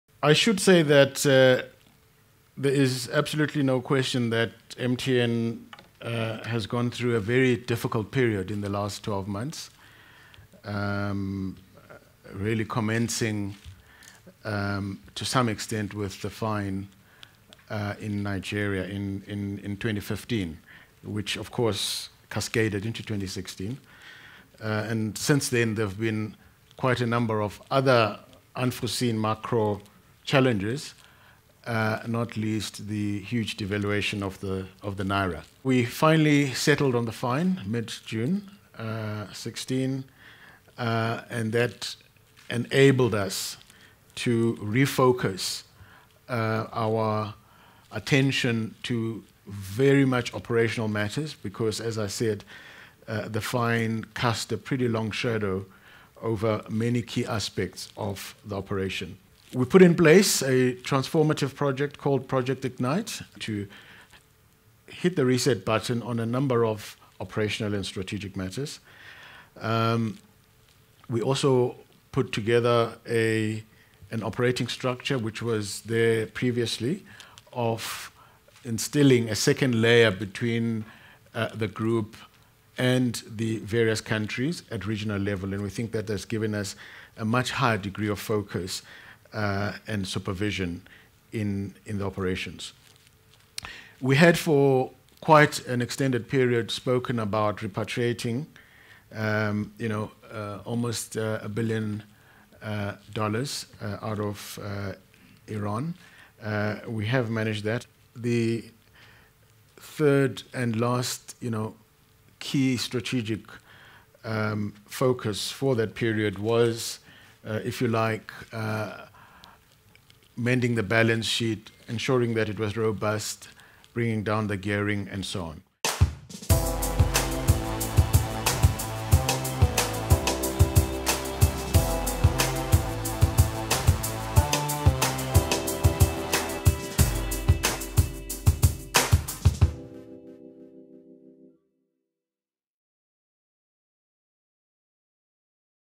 MTN optimistic of turnaround after plunging into the red in FY16
JSE-listed MTN believes it is on the mend and is seeing the first signs of a turnaround after plunging into the red during what it described as the most challenging year in the company's 22-year history.

With its Ignite initiative and various other strategies in play, MTN hopes to reverse the impact of a series of material regulatory, macroeconomic and political challenges experienced across its regions, which forced the company into a decline during the year ended December 31.